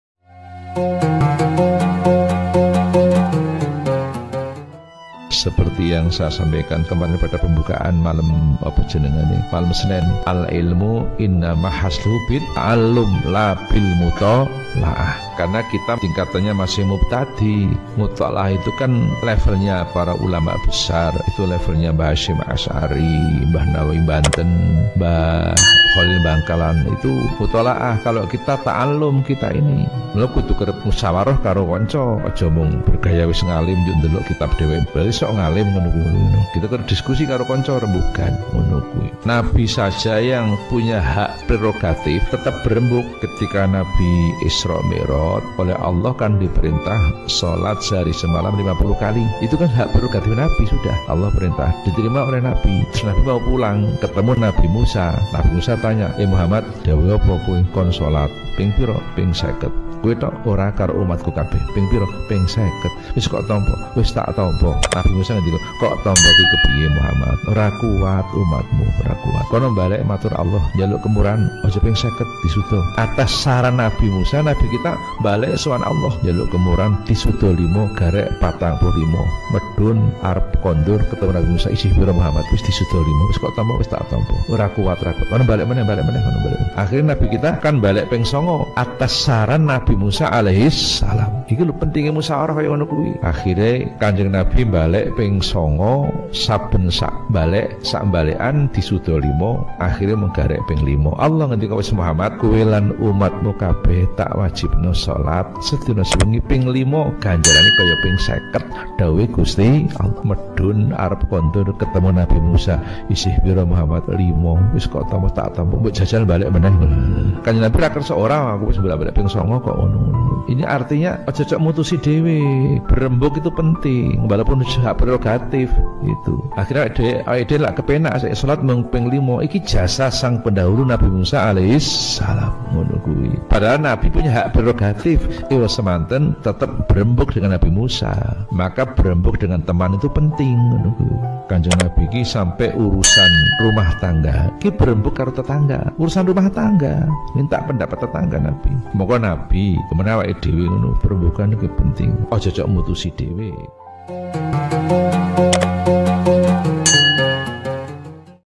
KH ACHMAD CHALWANI NAWAWI PENTINGNYA MUSYAWARAH WALAUPUN SUDAH HAK PREROGATIF
Apa arti pentingnya musyawarah? Mengapa musyawarah penting untuk mengambil keputusan? dan Apa pentingnya musyawarah dalam kehidupan bermasyarakat?
Nabi muhammad saja ketika isra' mi'ra bermusyawah dengan nabi musa

Arab Digarap